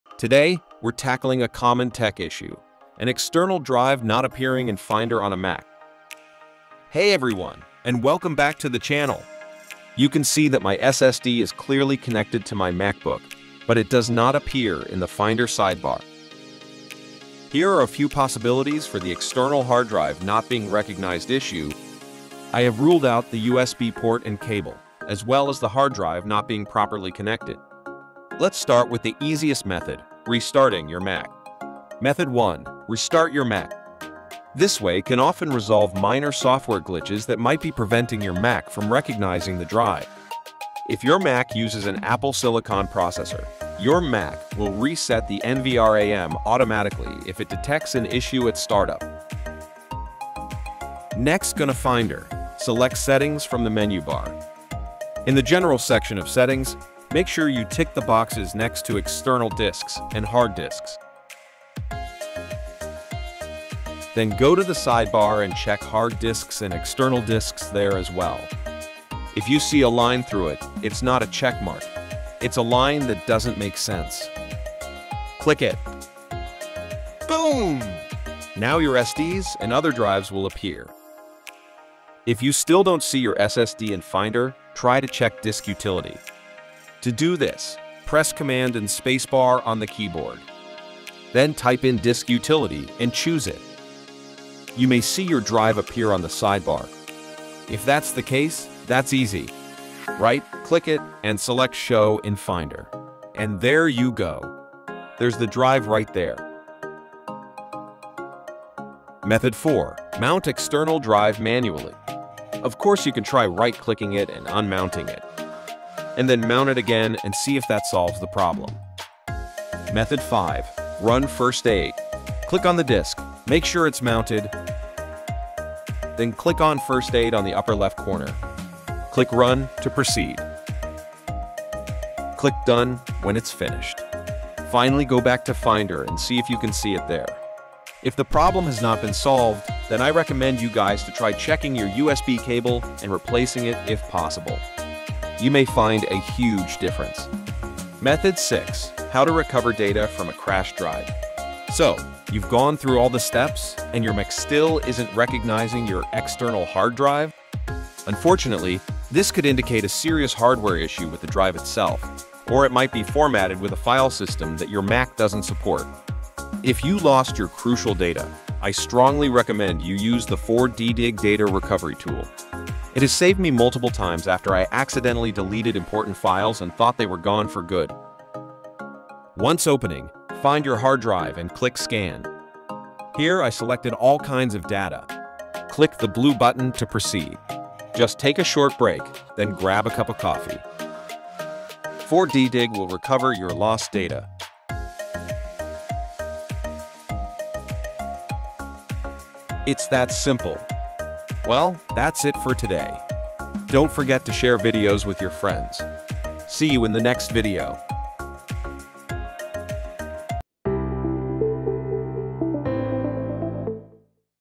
How to Fix External Hard Drive Not Showing Up on Mac? [6 Methods]
Having trouble with your external drive not showing up in Finder on your Mac? In this video, we’ll guide you through simple troubleshooting steps.

📌Timestamps:
00:00 Intro
00:32 Method 1: Restart Your Mac
00:49 Method 2: Edit Finder Settings
01:26 Method 3: Check Disk Utility
01:49 Method 4: Mount External Drive Manually
02:00 Method 5: Run First Aid
02:29 Method 6: How to Recover Data from A Crashed Drive

Quick Checklist Before Starting:
Confirm your external drive is powered (if it requires an external power source).
Verify the drive works on another device to rule out hardware issues.
Ensure your MacOS is up-to-date, as outdated software may cause compatibility problems.